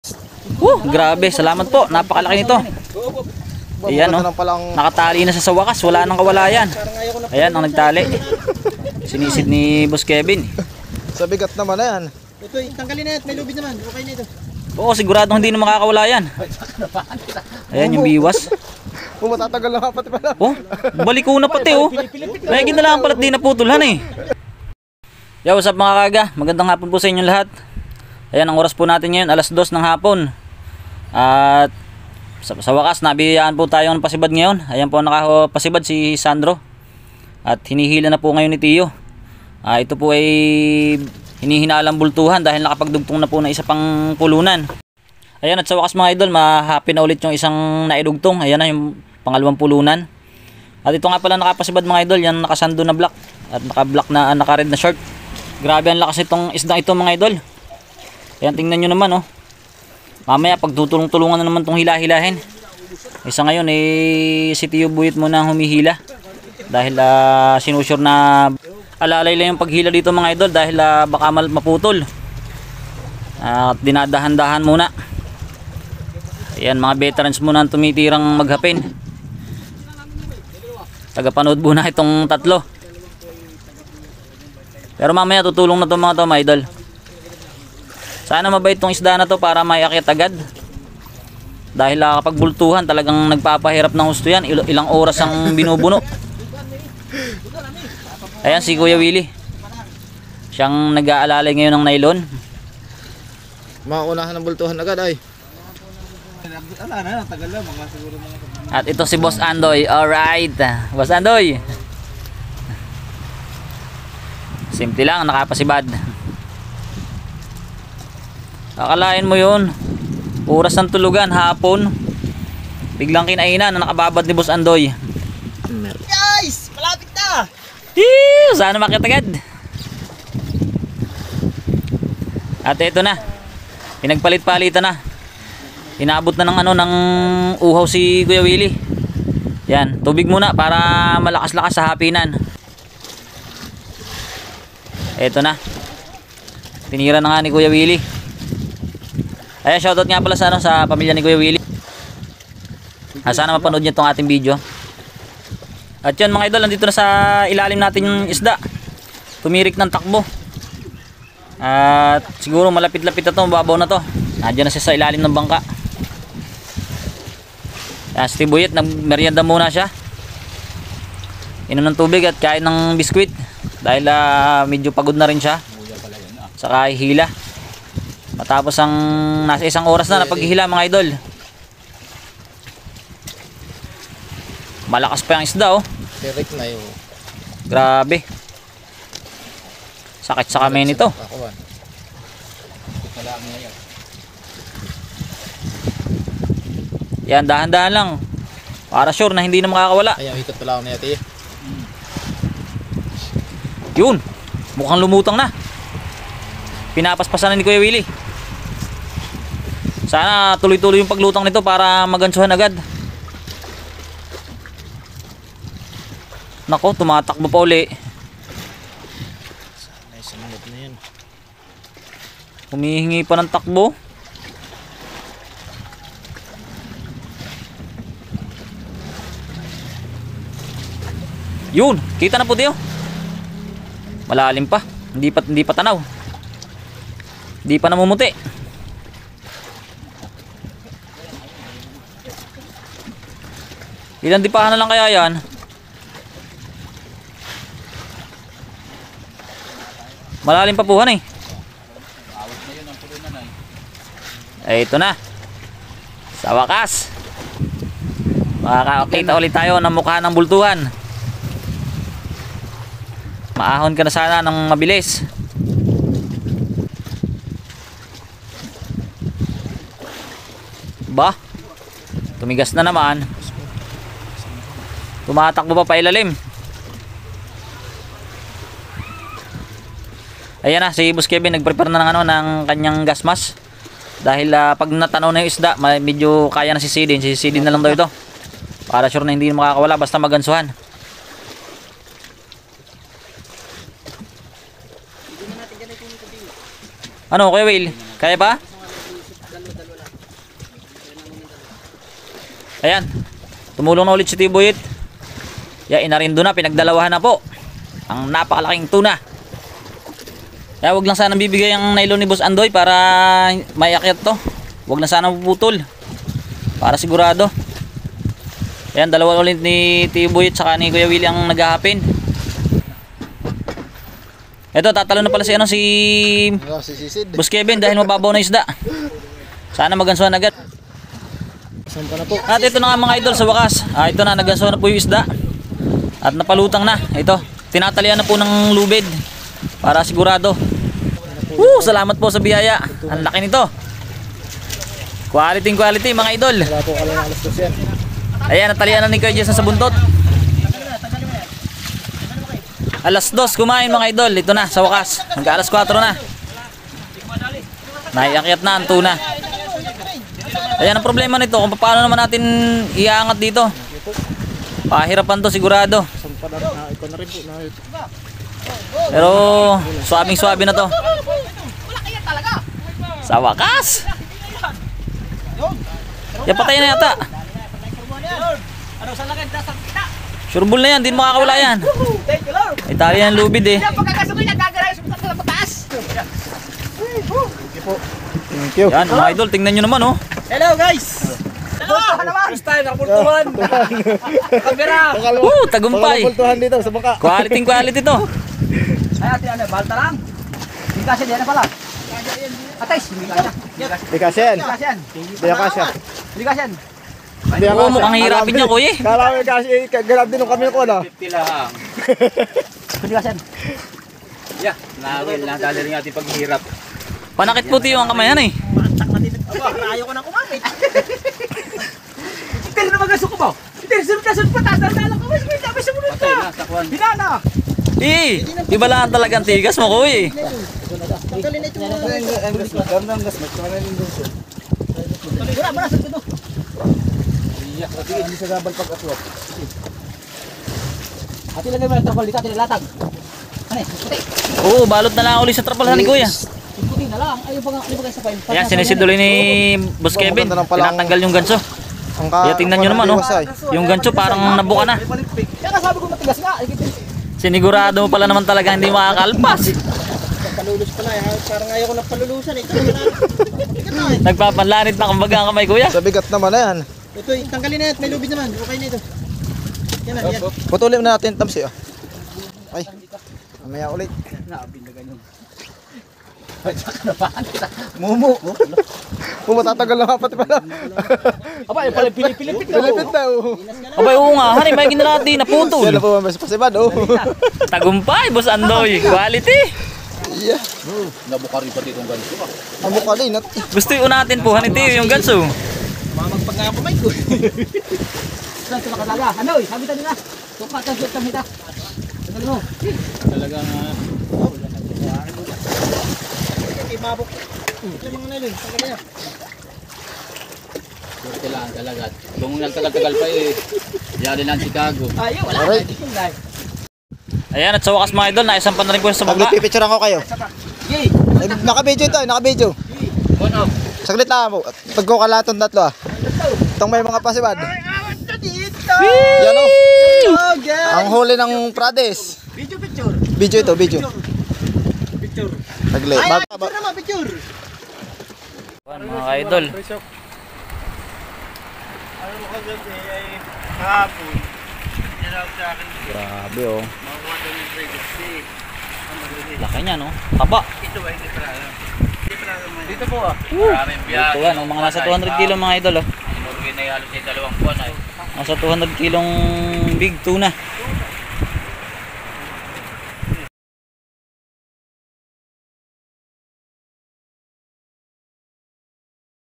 Dalawang Oras na Pakikipag hilahan sa Giant Bluefin Tuna sa Pacific Ocean
Halos Masugat Ang mga kamay sa Pag Hila sa Dambuhalang Tuna